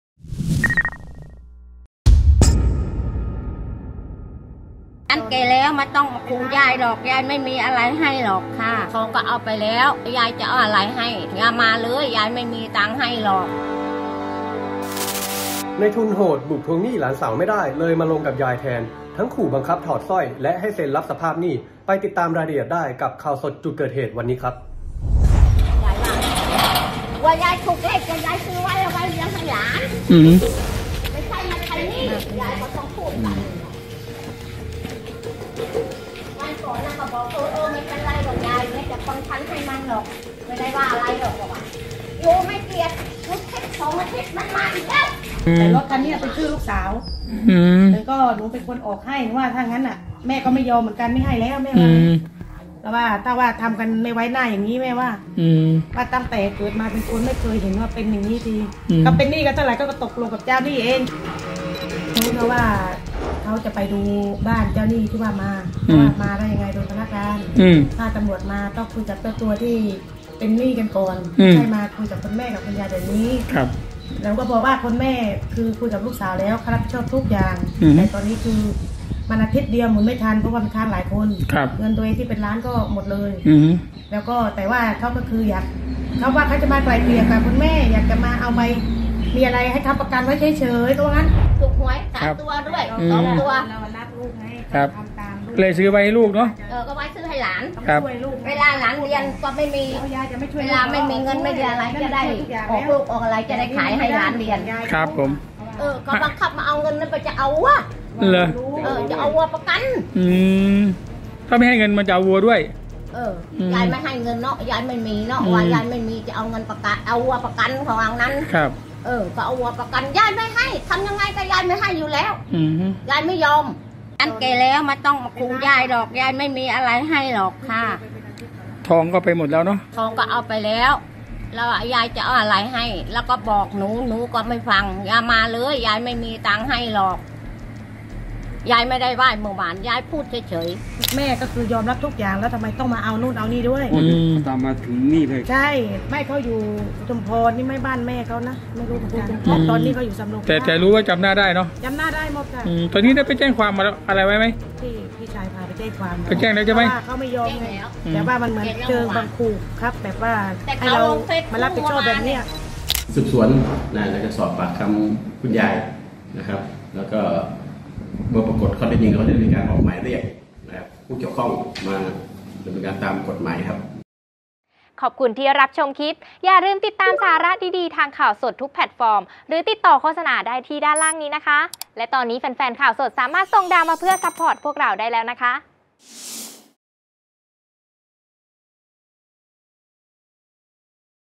ยายกราบขอร้อง ไม่มีเงินให้แล้ว เจอนายทุนเงินกู้โหด ให้ใช้หนี้แทนหลาน : Khaosod TV
ยายกราบขอร้อง ไม่มีเงินให้แล้ว เจอนายทุนเงินกู้โหด ยึดสร้อยทอง ขู่ให้รับสภาพหนี้แทน หลานสาว